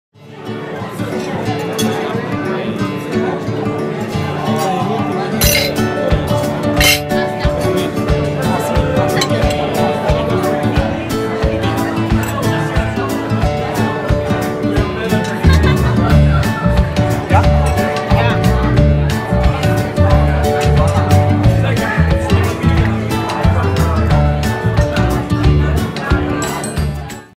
Cumpleaños de Renzo Peralta Parte # 2 "Ristorante Barbacoa"
Dove : - Milano /Italia
Quando : 25/11/2016
Presenti : Alfredo Peralta,Renzo Peralta,Shakira Peralta,Clarita Bedon.